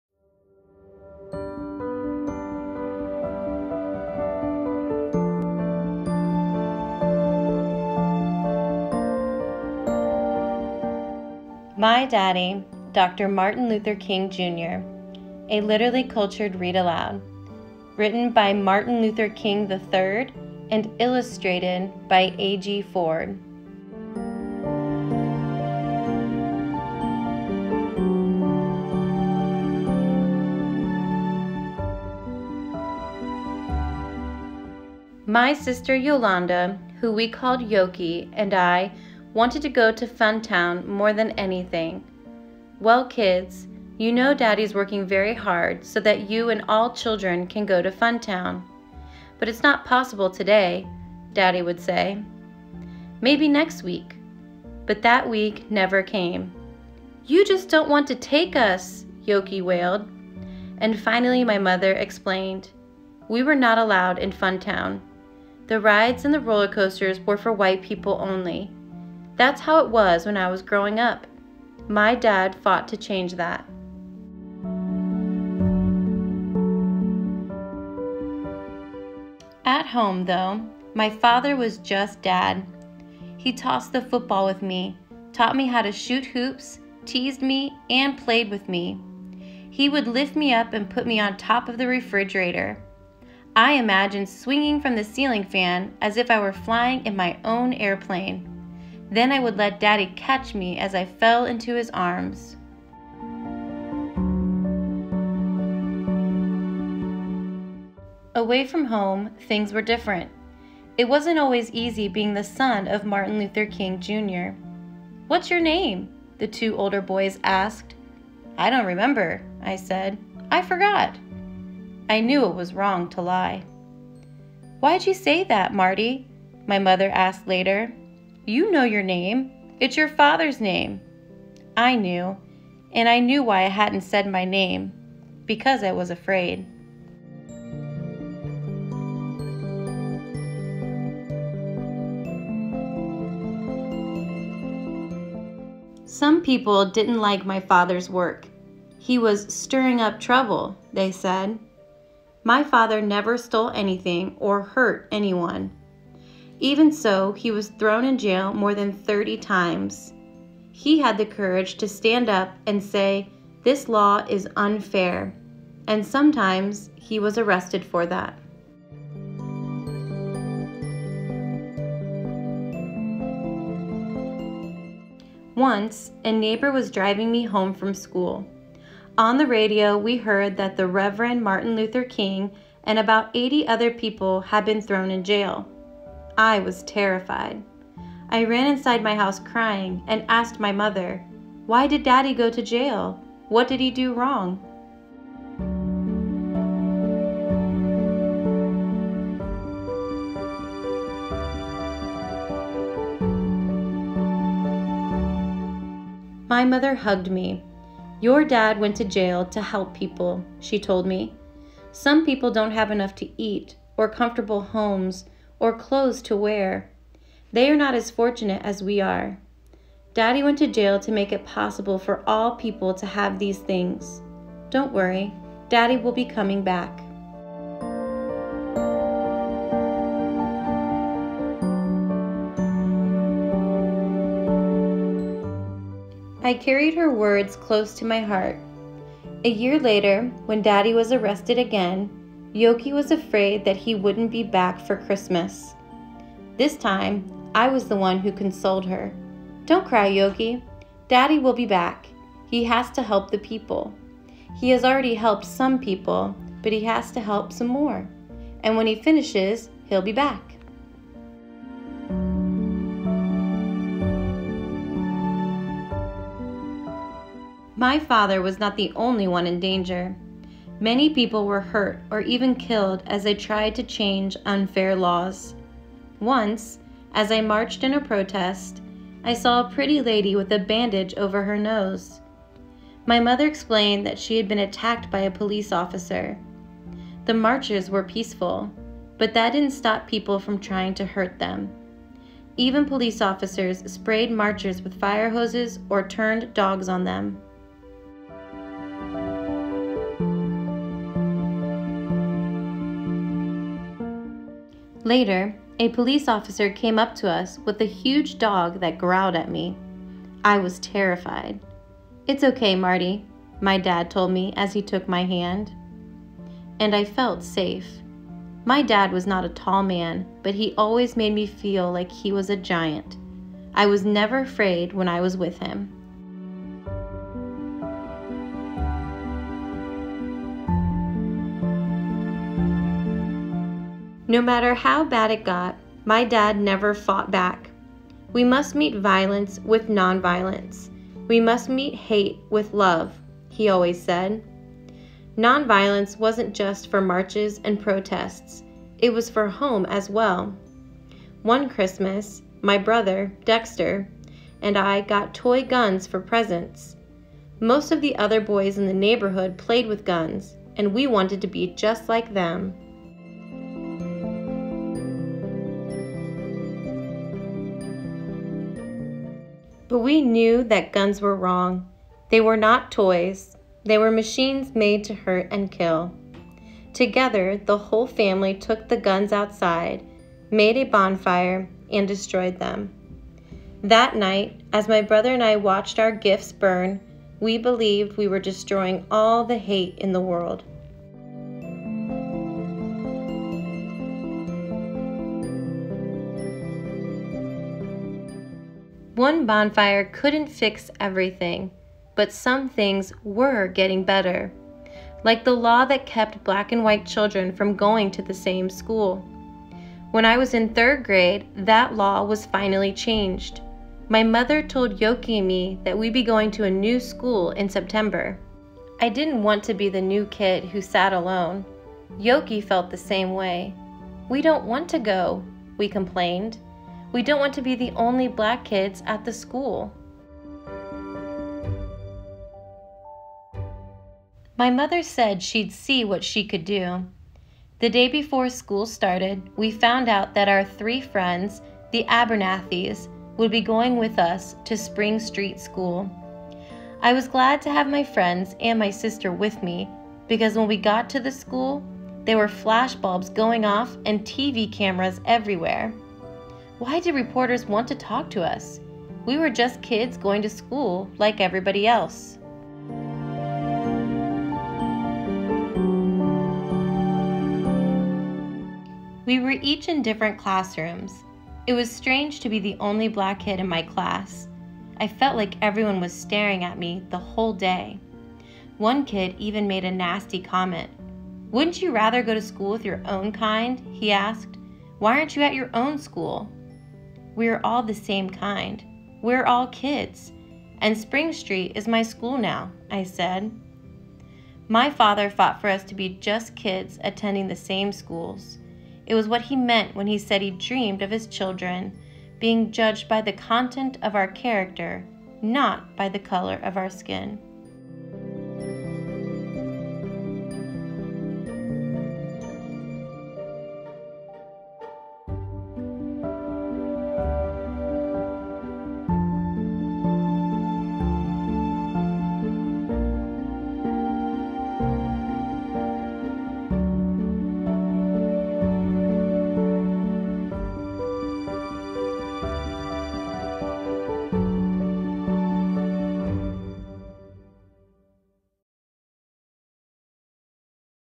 My Daddy, Dr. Martin Luther King, Jr. (Literally Cultured Read Aloud)
Title: My Daddy, Dr. Martin Luther King, Jr.
Author: Martin Luther King III
Illustrator: AG Ford
Publisher: Amistad
Publication: January 2, 2018
Narrator: Shannon Griffin

“I have a dream that my four little children will one day live in a nation where they will not be judged by the color of their skin but by the content of their character.”

Martin Luther King III was one of those four little children mentioned in Martin Luther King’s groundbreaking “I Have a Dream” speech. In this memoir, Martin Luther King Jr.’s son gives an intimate look at the man and the father behind the civil rights leader. Mr. King’s remembrances show both his warm, loving family and a momentous time in American history.